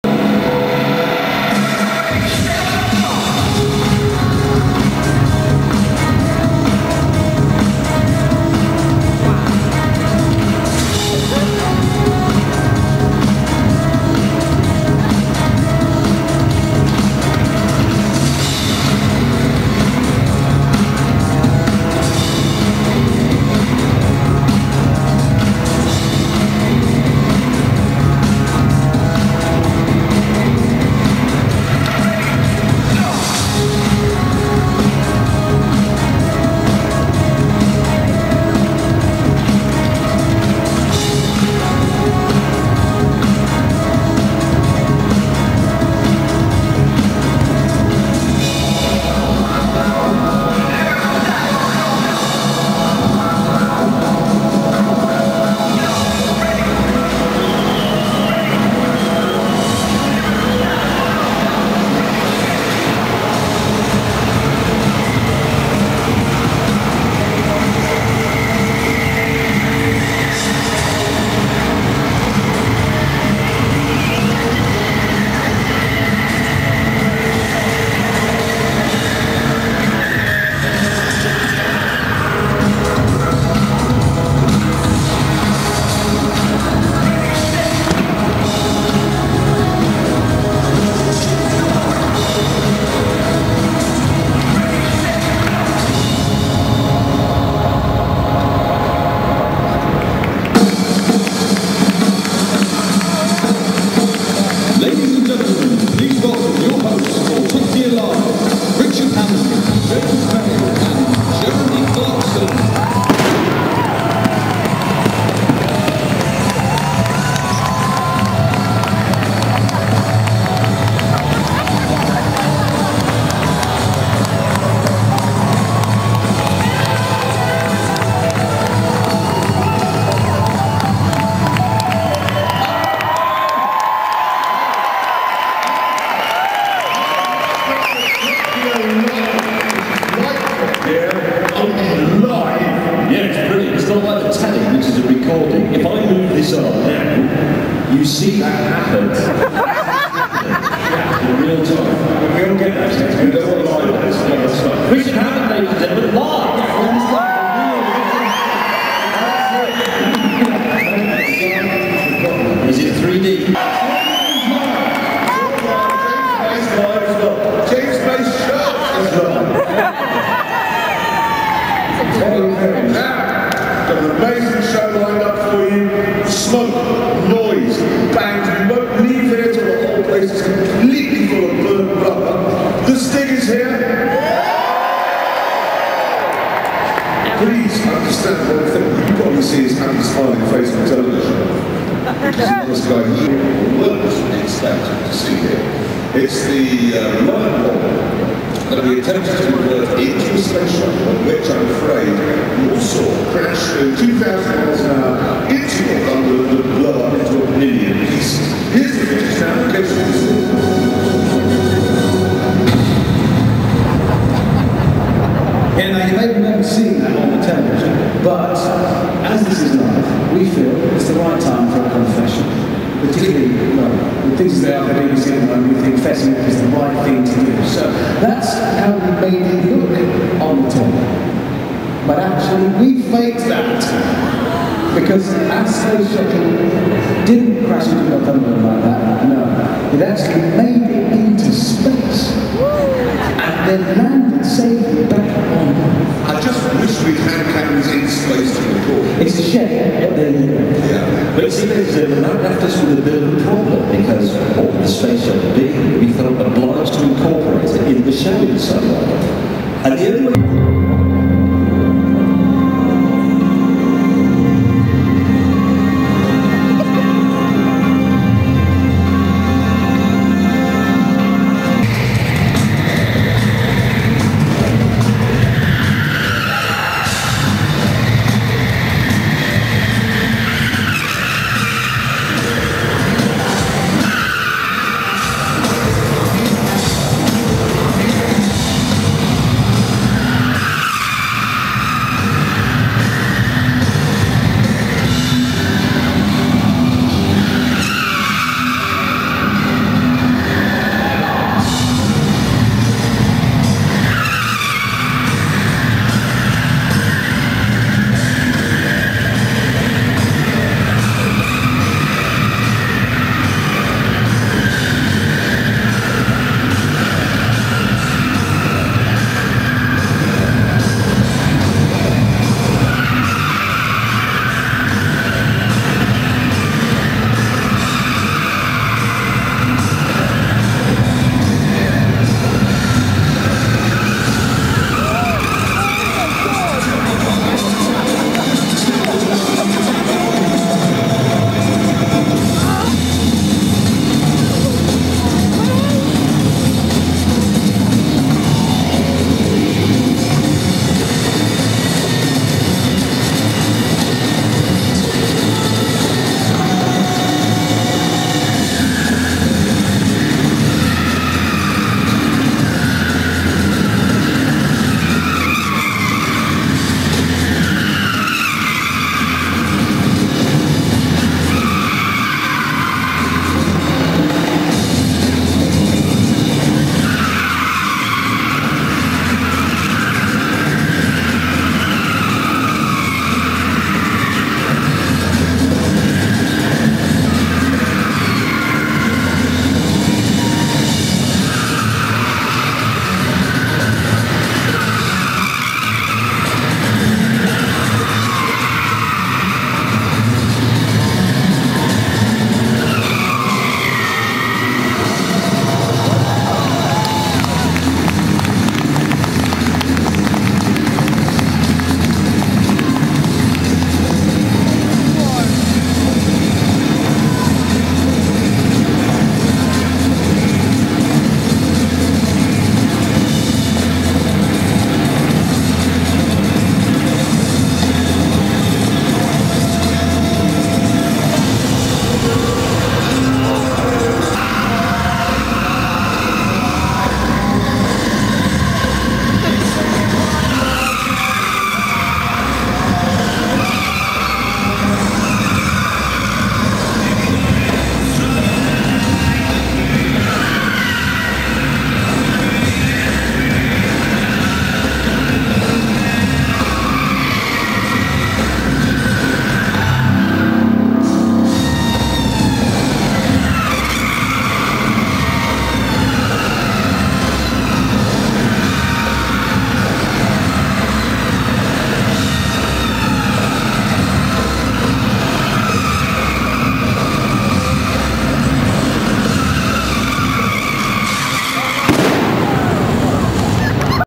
Top Gear Live! #1
Random bits from the beginning of Top Gear Live!
Bham Nec 16/11/2008
soz about sum of the shakey cam work, my arm started to die :)
plus watch out for the insane bursts of laughter, the woman next to me was kinda crazy for clarkson.
Enjoy, more bits coming soon...